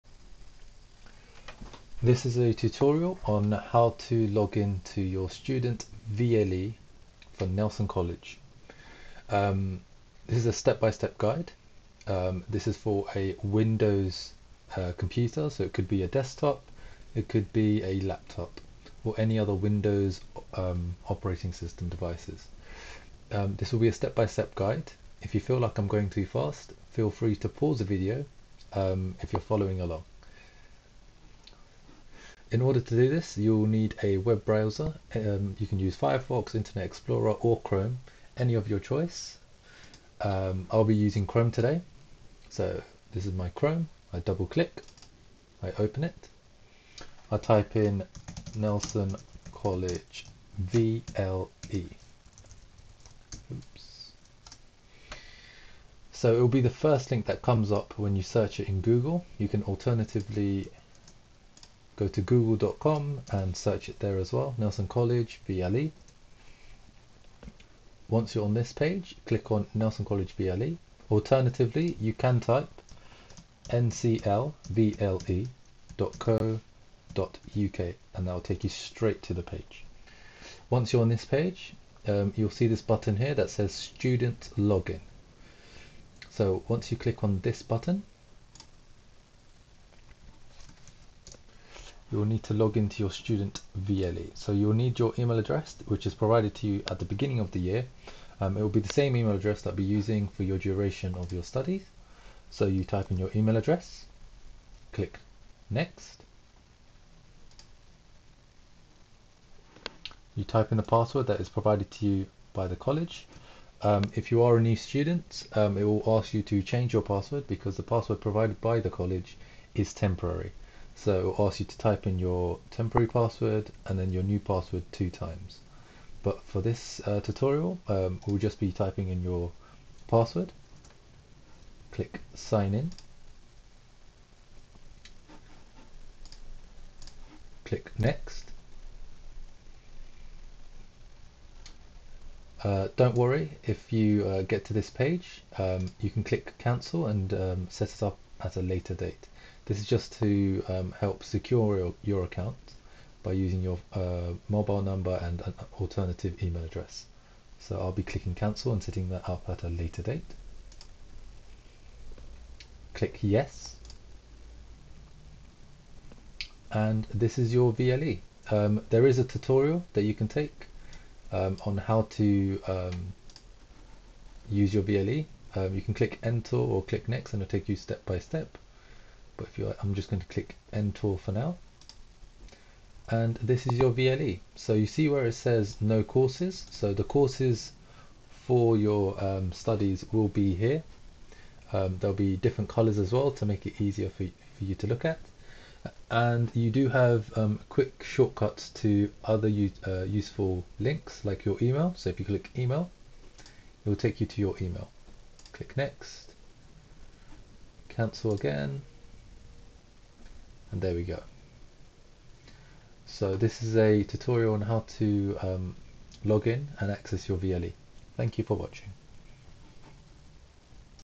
NCLVLE Login
This video is part of a series of videos for students of Nelson College London, helping them with their understanding of the College's learning facilities, which are used as the base for online lessons and remote access.

This video focusses on using the VLE (Virtual Learning Environment) and how to log on to it, on a Windows device.  This should also inform users of Apple devices, however, Safari does not always work well as a browser for this purpose, so users may need to install Chrome.